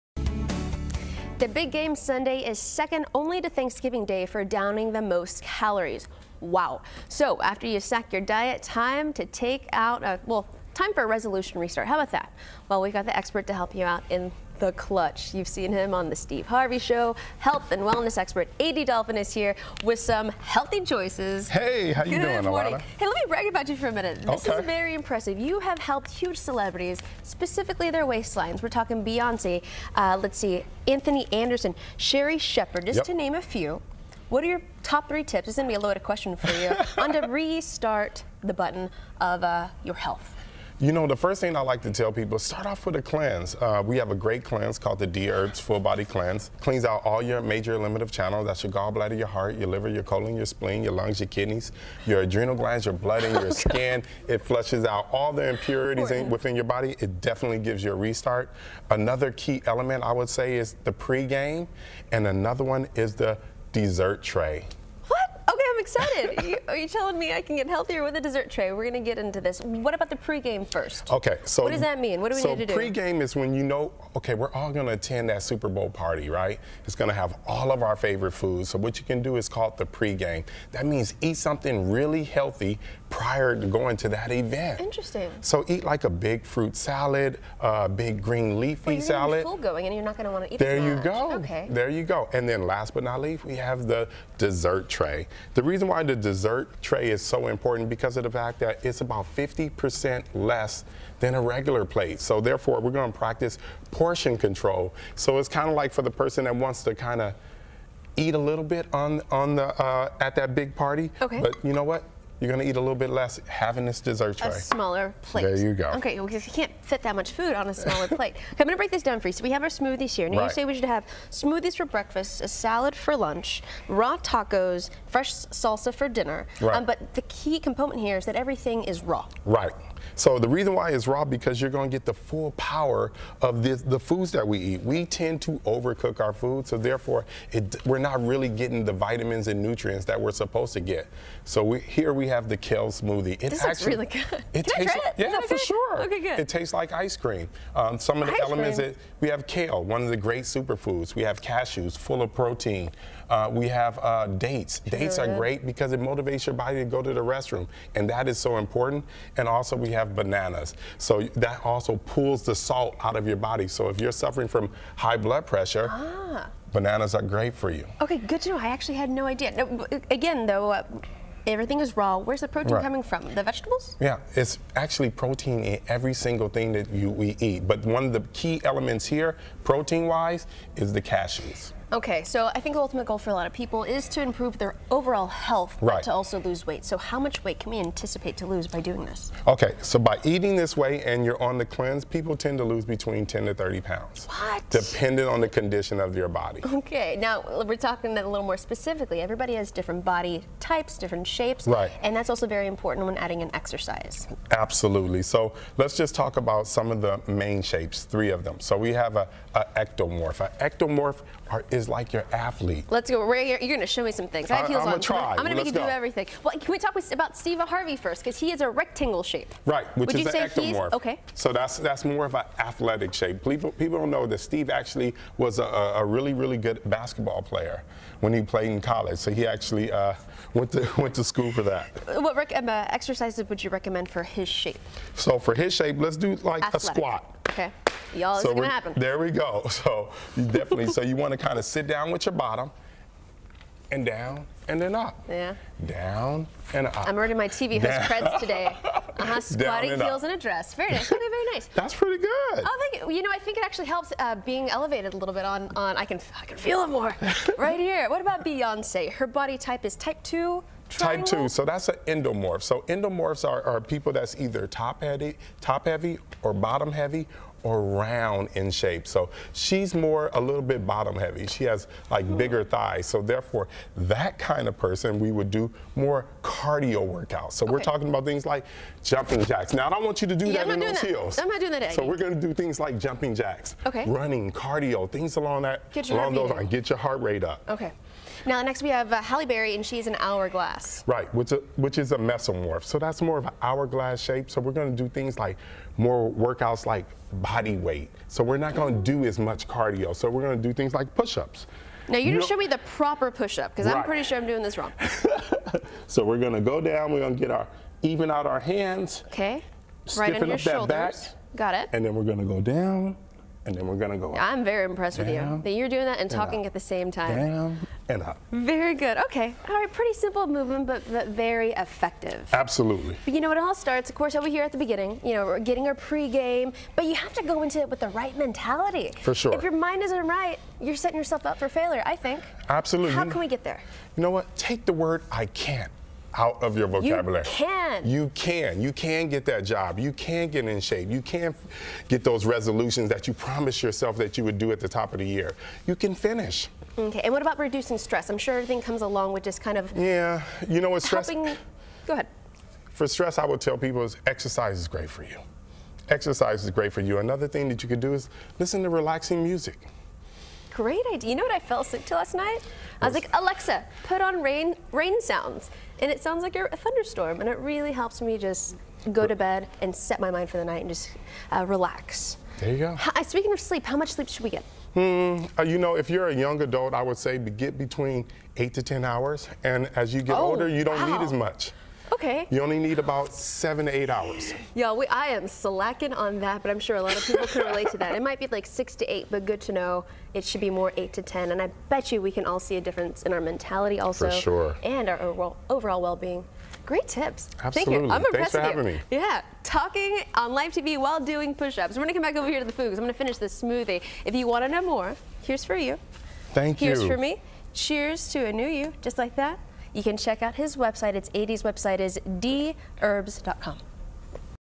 Restart Your Resolutions with these Tips from AD Dolphin | Dherbs
Health and Wellness Expert, AD Dolphin, went down to Texas to help them out with a Resolution Reset! It's been a month since we all started with our New Years' Resolutions and it's time to check back in. Have you let them drop or are you on your way to your goal? Progress is progress.